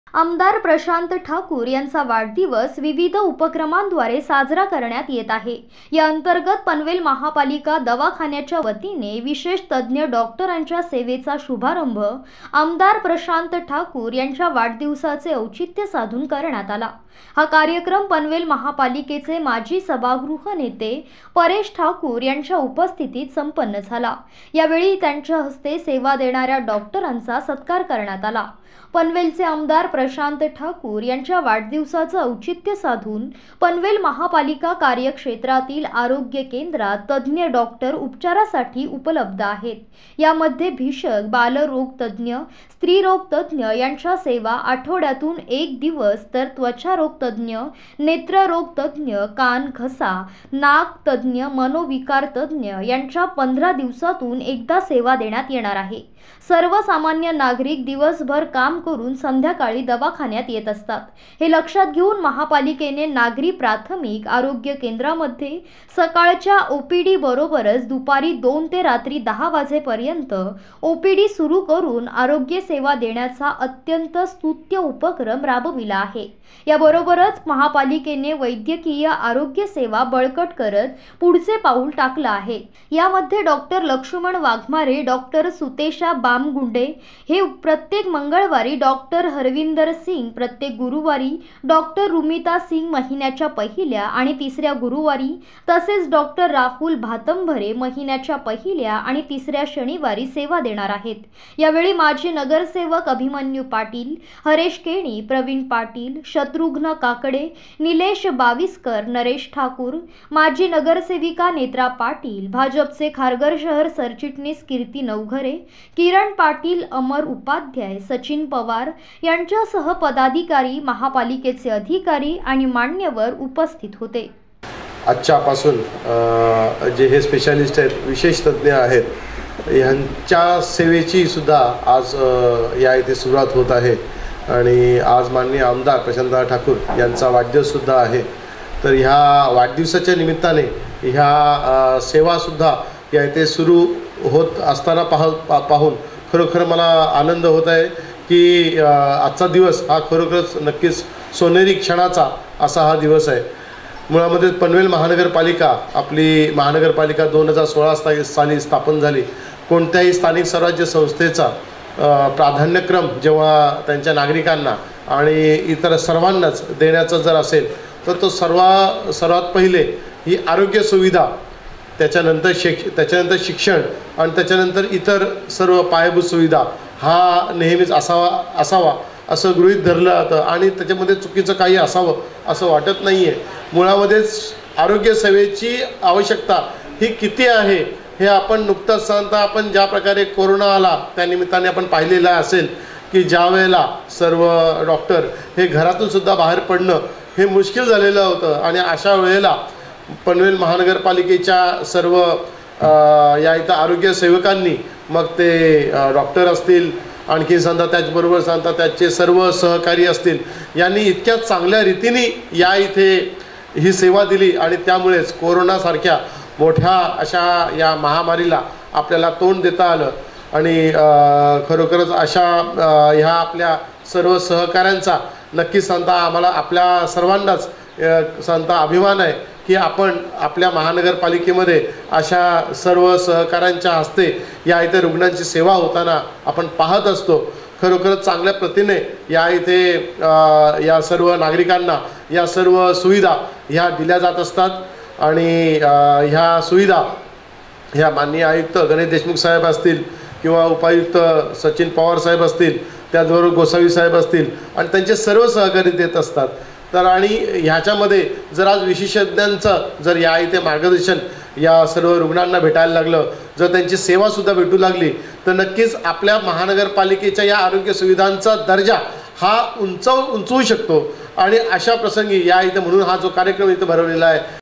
लोकप्रिय आमदार प्रशांत ठाकूर यांच्या वाढदिवसानिमित्त मोठ्या प्रमाणात विविध सामाजिक उपक्रमे
आमदार प्रशांत ठाकूर यांचा वाढदिवस विविध उपक्रमांद्वारे साजरा करण्यात येत आहे. याअंतर्गत पनवेल महापालिका दवाखानाच्या वतीने विशेष तज्ञ डॉक्टरांच्या सेवेचा शुभारंभ आमदार प्रशांत ठाकूर यांच्या वाढदिवसाचेऔचित्य साधून करण्यात आला आहे. हा कार्यक्रम पनवेल महापालिकेचे माजी सभागृह नेते परेश ठाकूर यांच्या उपस्थितीत संपन्न झाला. यावेळी त्यांच्याहस्ते सेवा देणाऱ्या डॉक्टरांचा सत्कार करण्यात आला.